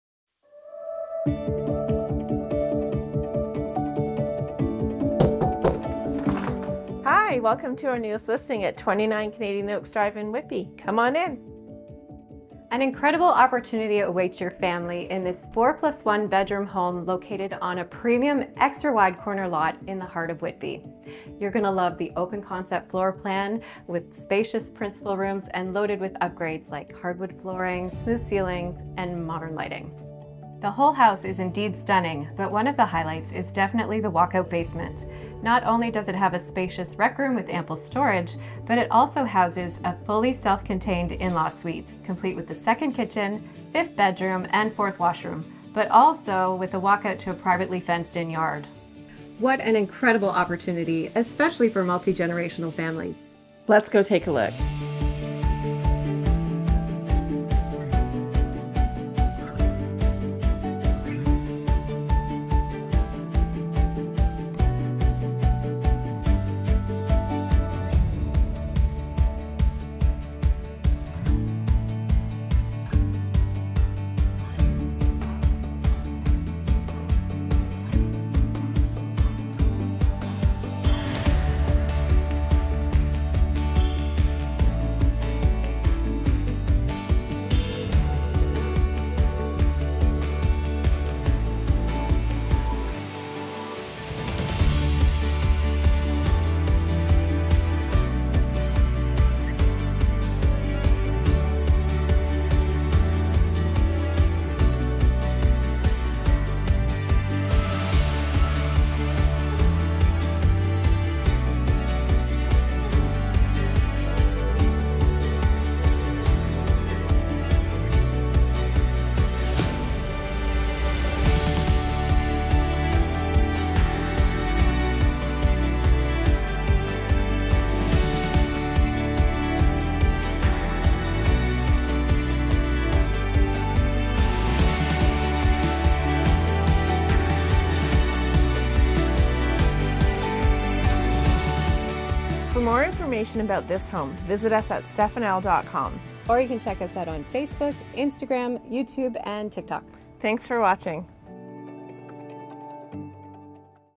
Multi-generational Family? No Problem!!
🔥JUST LISTED!🔥
29 Canadian Oaks Drive, Whitby
$1,099,900

An incredible opportunity awaits for your family in this absolutely gorgeous 4+1 bedroom executive home located on an extra-wide corner lot with walk-out basement and fabulous in-law suite! Enjoy a meticulously kept, bright & open-concept floor plan with large principal rooms loaded with upgrades like smooth ceilings, pot lights & modern lighting and hardwood flooring. Chef's kitchen with maple cabinetry, gas stove and granite counters and a cozy family room offering a gas fireplace and walk-out to the beautiful deck overlooking the expansive yard with above-ground pool - perfect for the upcoming summer swim season! Formal living/dining rooms for easy entertaining and a stunning laundry room with garage access! Enjoy 4 spacious well-decorated bedrooms upstairs with two beautifully updated bathrooms. The finished basement offers a rec room, gym and large storage  plus an incredible in-law suite. With a 2nd kitchen, 5th bedroom, family room and private, fully-fenced yard with a private deck, the options are endless and it truly is the perfect spot for extended families! Excellent location close to all amenities in a neighbourhood that has true pride of ownership, mature trees and beautiful parks! Don't miss it!

 • Executive brick home on a premium 89ft wide corner lot!
 • 4+1 bedrooms, 4 bathrooms
 • Finished walk-out basement with rec room, storage room AND fully contained in-law suite with 2nd kitchen, 5th bedroom and bathroom plus walk-out to fully fenced, private yard and deck!
 • Above-ground pool with liner ’16 and solar blanket
 • Modern kitchen with maple cabinetry and granite counters
 • Renovated bathrooms
 • Hardwood flooring, smooth ceilings, modern lighting and potlights
 • Furnace & hot water tank ’23, Fridge ’23, Roof ’21, A/C ’14, Decks ‘14
 • Excellent location close to parks, schools, all amenities and easy Hwy access!

RE/MAX Rouge River Realty Ltd

itby